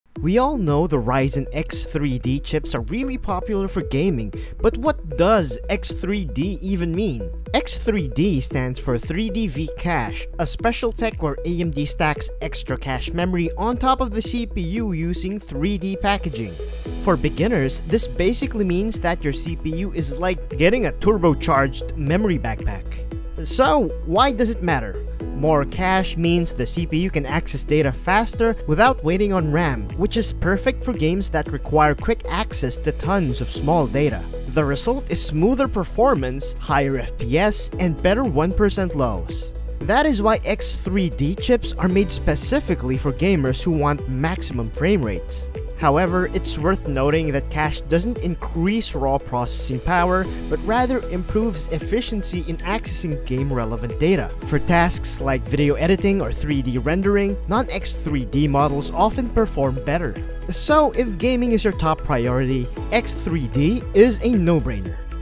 What Does X3D Mean in Ryzen CPUs
We all know that Ryzen X3D chips are really popular for gaming, but what does ‘X3D’ even mean?

X3D stands for 3D V-Cache—a special tech where AMD stacks extra cache memory on top of the CPU using 3D packaging.

For beginners, this basically means that your CPU is like getting a turbocharged memory backpack.

So, why does it matter?

More cache means the CPU can access data faster without waiting on RAM, which is perfect for games that require quick access to tons of small data.

The result is smoother performance, higher FPS, and better 1% lows.

That is why X3D chips are made specifically for gamers who want maximum frame rates.

However, it’s worth noting that cache doesn't increase raw processing power, but rather improves efficiency in accessing game-relevant data.

For tasks like video editing or 3D rendering, non-X3D models often perform better.

So, if gaming is your top priority, X3D is a no-brainer.